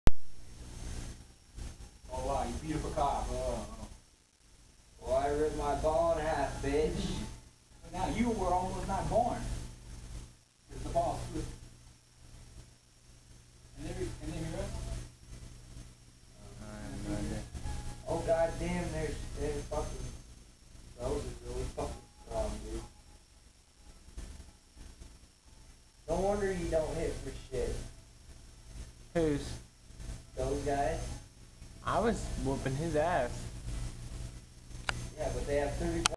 Xivilai Chilling
I killed a Xivilai. I think that's what their called. and it happebd to fall and land like this. I think he looks like he is chillaxed on the ground just chilling.